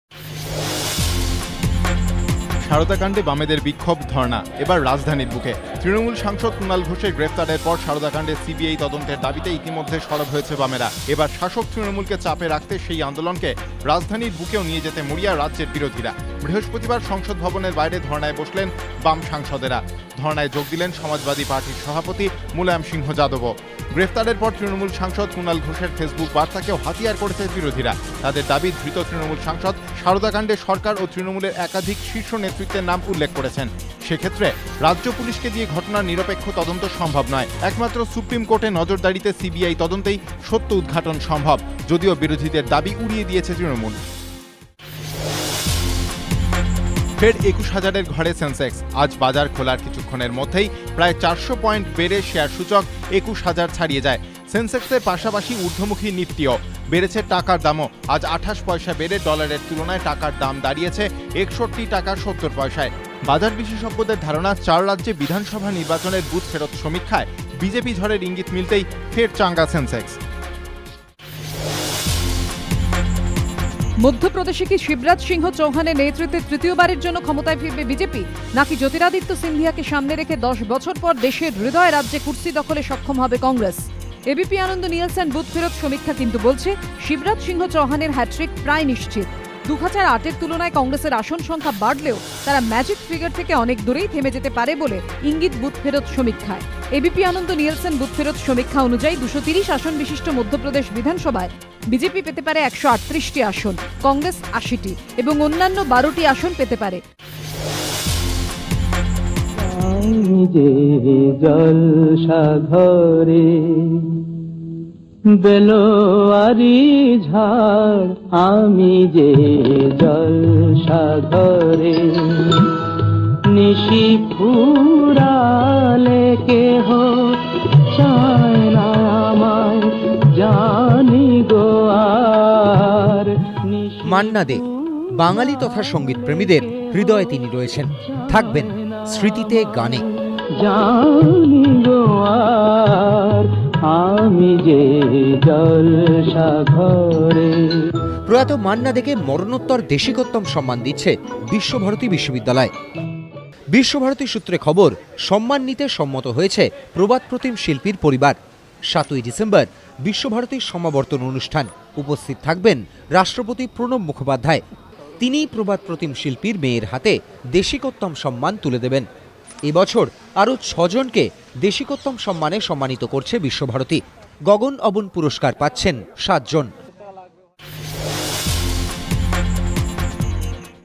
3 মিনিটে এই মুহুর্তের বড় খবর
Left MPs in Dharna outside parliament demanding CBI probe in Saradha scam,